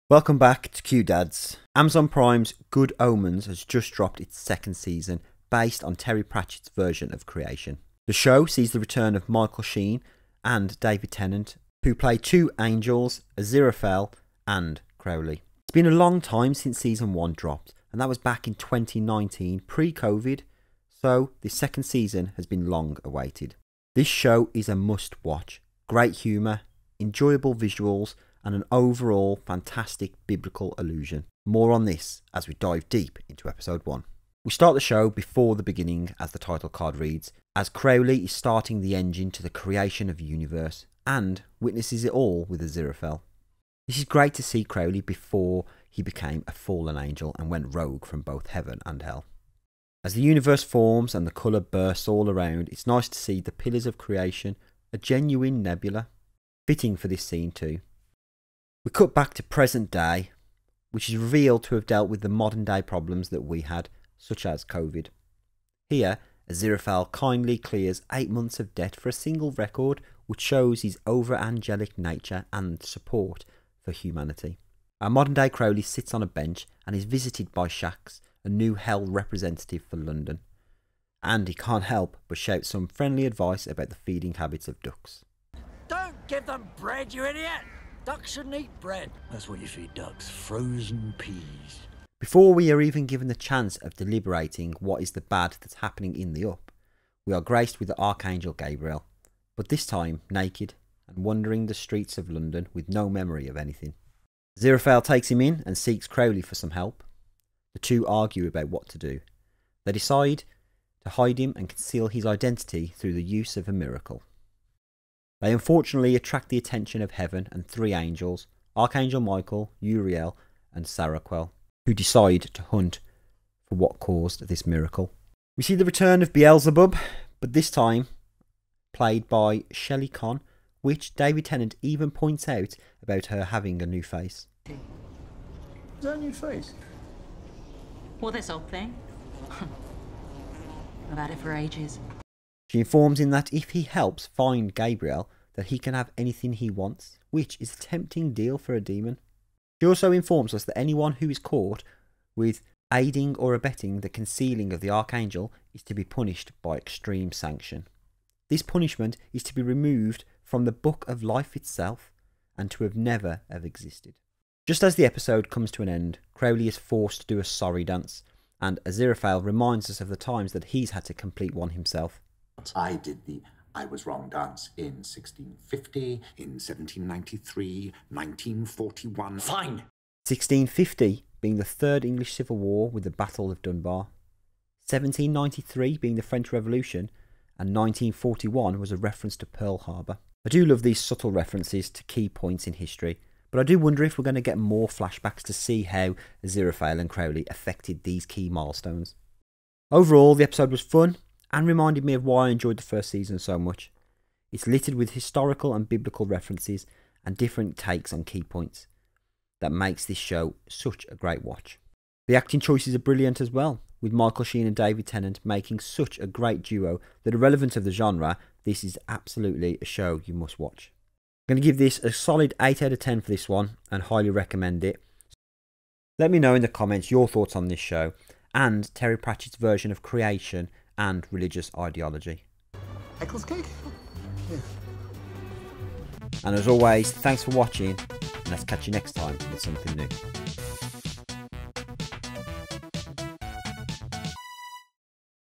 Good Omens Season 2 Episode 1 Review and Breakdown "The Arrival" | Amazon Prime
Join us as we review, recap and breakdown Good Omens season 2 episode 1, “The Arrival".

Episode 1 Plot:

Retired angel Aziraphale and retired demon Crowley's lives are upended when a visitor arrives on the doorstep of Aziraphale's bookshop. Local shopkeepers Maggie and Nina get locked in to Nina's coffee shop when Crowley loses his temper.